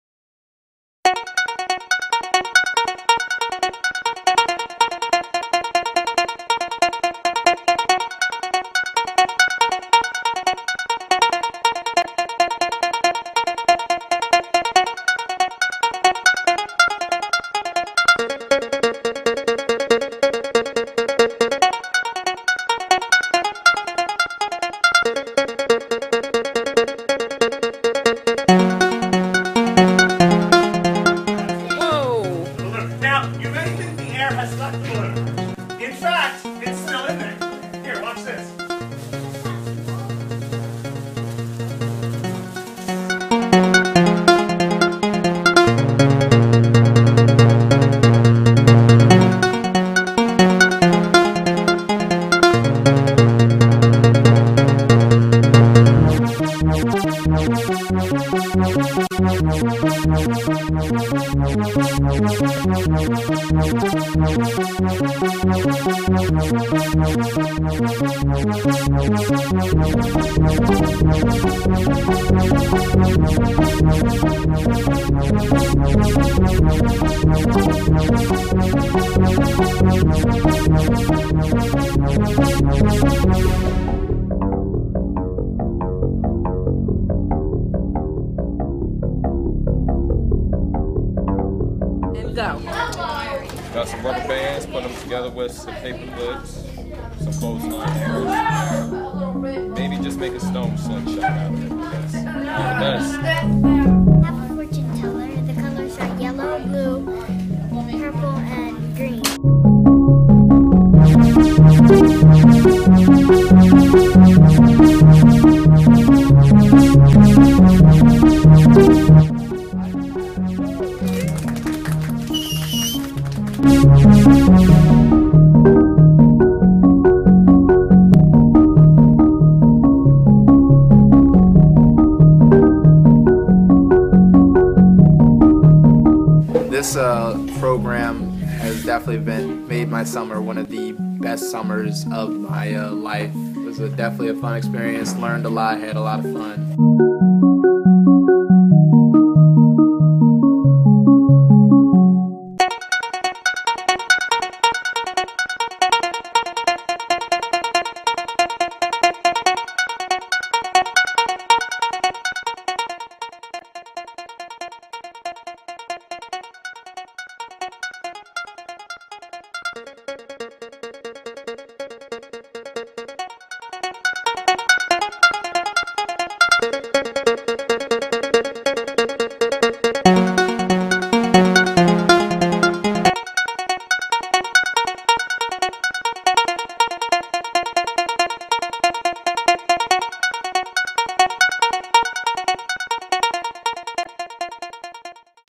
Lansdowne SRC'12
Lansdowne Librarian's Rachee Fagg and Abbe Klebanoff wish to thank all our volunteers, staff, The Friends of The Lansdowne Library and the Lansdowne Allied Youth Council for making Summer Reading 2012 a success. More than 2671 kids and teens participated in our 128 programs and more than 1,700 books were read. We gave away an iPad and the children's "book store" opening was met with great excitement. Again, thanks to all those who made this summer possible.